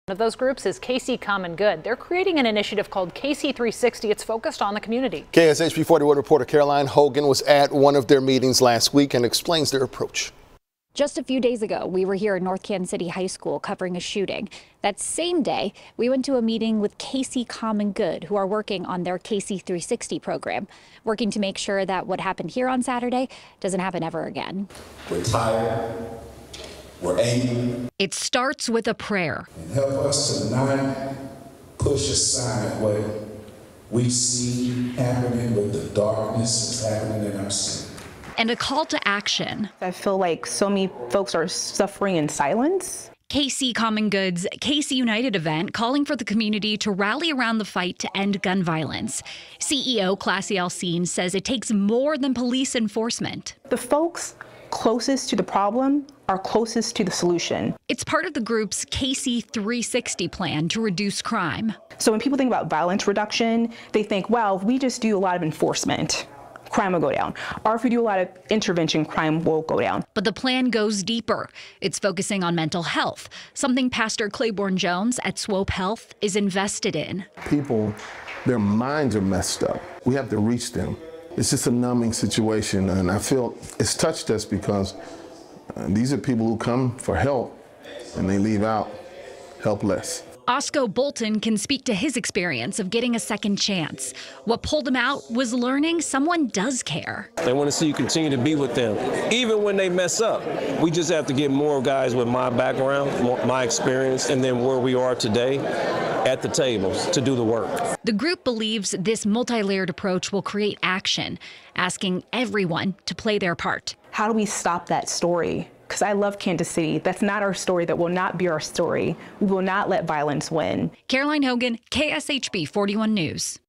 KC Common Ground rallies community members, organizations to fight gun violence
KC Common Ground is rallying community members, organizations to fight gun violence.